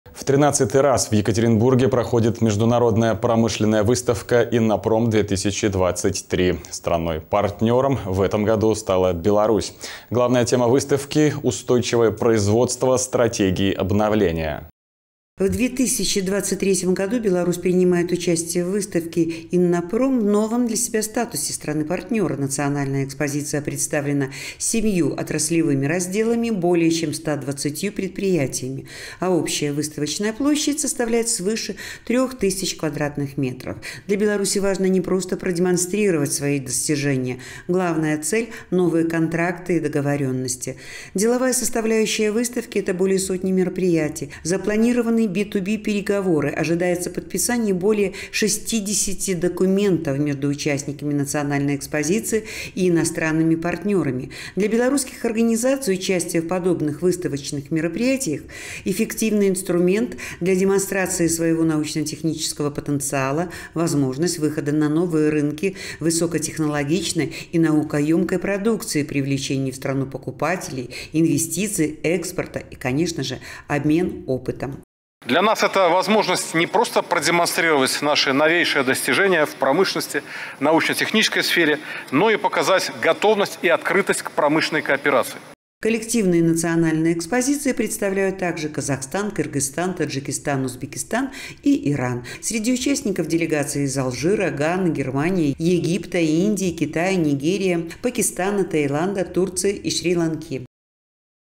2023-07-12 Международная промышленная выставка «Иннопром-2023»
В 13-ый раз в Екатеринбурге проходит Международная промышленная выставка «Иннопром-2023». Страной-партнером в этом году стала Беларусь. Главная тема выставки   — «Устойчивое производство: стратегии обновления».
*Наши контакты:*
✅Телефон горячей линии:
54-63-67